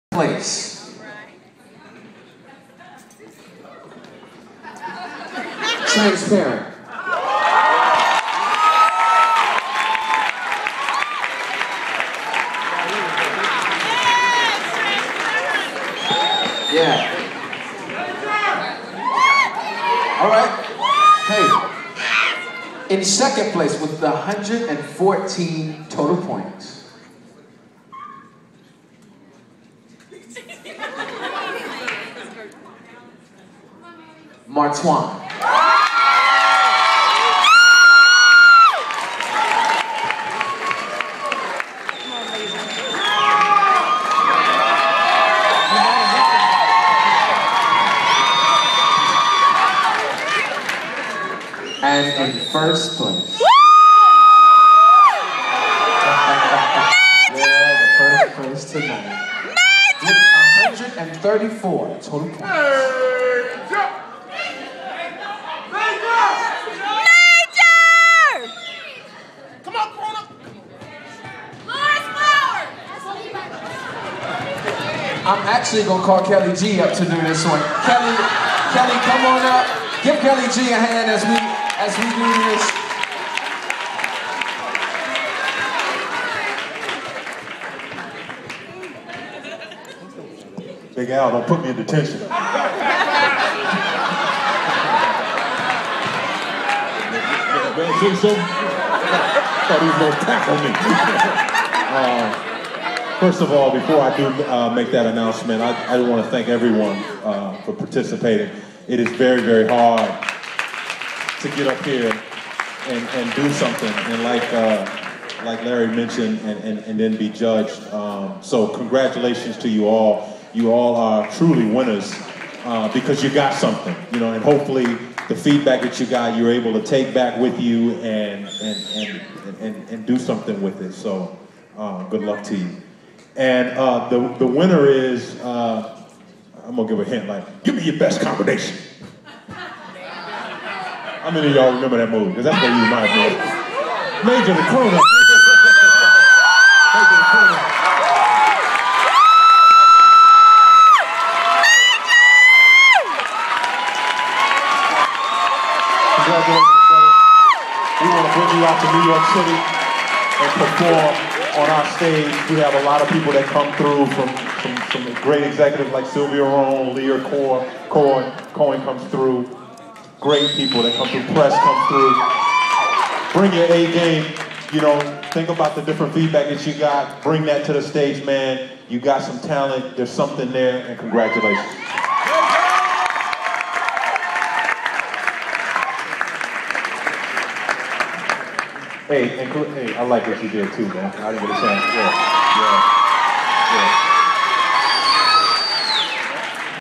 Major Crooner wins the BET Music Matters contest 2012
witness Major Crooner win the BET Music Matters contest at The Merge Summit in Los Angeles, Aug 24, 2012.

The audience was screaming his name before he was confirmed; evidence that he's a born star!!